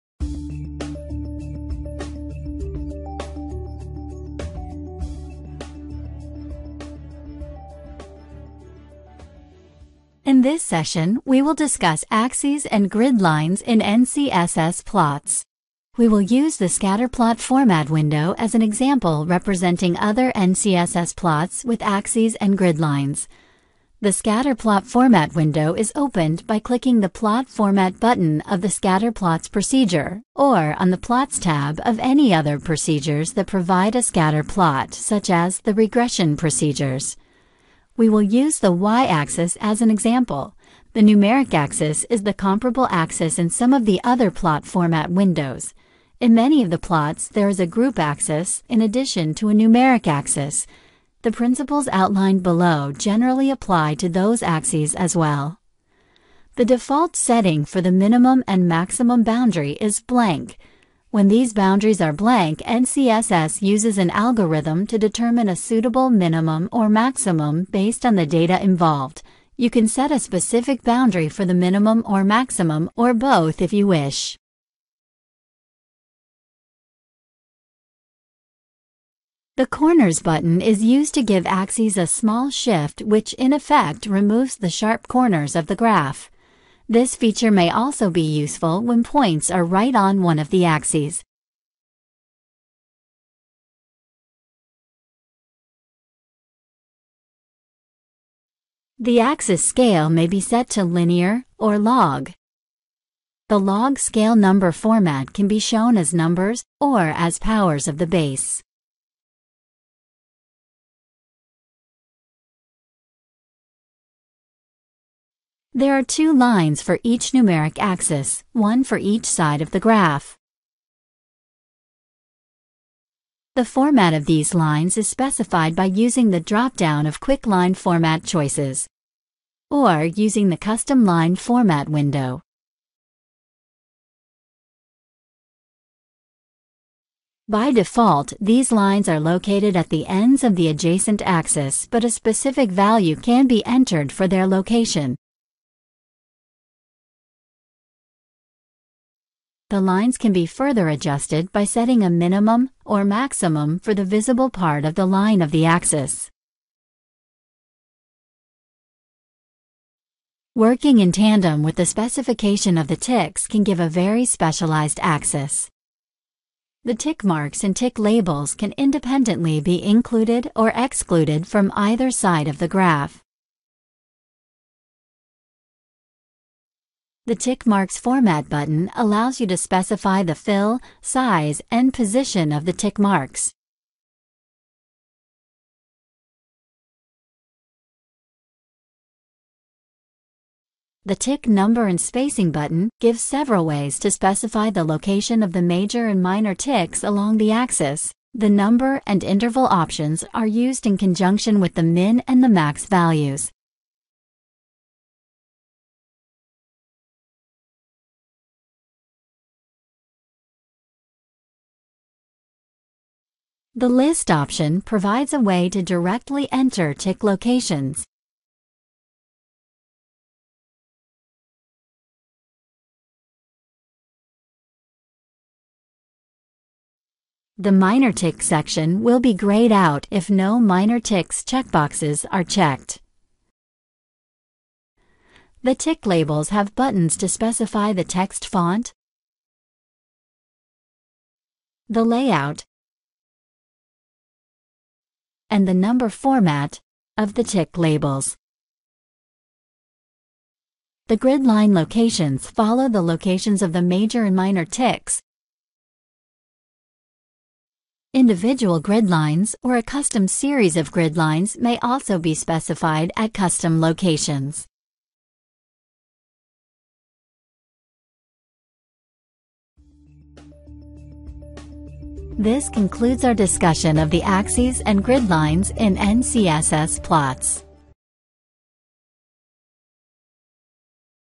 Specifying Plot Axes and Grid Lines in NCSS
In this tutorial, we will discuss axes and grid lines in NCSS plots.

We will use the scatter plot format window as an example representing other NCSS plots with axes and grid lines. The scatter plot format window is opened by clicking the plot format button of the scatter plots procedure, or, on the plots tab of any other procedures that provide a scatter plot, such as the regression procedures.

We will use the Y axis as an example. The numeric axis is the comparable axis in some of the other plot format windows. In many of the plots there is a group axis in addition to a numeric axis. The principles outlined below generally apply to those axes as well.

The default setting for the minimum and maximum boundary is 'blank'. When these boundaries are blank, NCSS uses an algorithm to determine a suitable minimum or maximum based on the data involved. You can set a specific boundary for the minimum or maximum or both if you wish.

The 'Corners' button is used to give axes a small shift, which, in effect, removes the sharp corners of the graph. This feature may also be useful when points are right on one of the axes.

The axis scale may be set to linear or log.  The log scale number format can be shown as numbers or as powers of the base.

There are two lines for each numeric axis, one for each side of the graph.  The format of these lines is specified by using the drop-down of quick line format choices, or using the custom line format window. By default, these lines are located at the ends of the adjacent axis, but a specific value can be entered for their location. The lines can be further adjusted by setting a minimum or maximum for the visible part of the line of the axis. Working in tandem with the specification of the ticks can give a very specialized axis.

The tick marks and tick labels can independently be included or excluded from either side of the graph. The tick marks format button allows you to specify the fill, size, and position of the tick marks. The 'Tick Number and Spacing' button gives several ways to specify the location of the major and minor ticks along the axis. The number and interval options are used in conjunction with the Min and the Max values. The 'List' option provides a way to directly enter tick locations. The minor tick section will be grayed out if no minor ticks checkboxes are checked.

The tick labels have buttons to specify the text font, the layout, and the number format of the tick labels.

The grid line locations follow the locations of the major and minor ticks. Individual grid lines or a custom series of grid lines may also be specified at custom locations.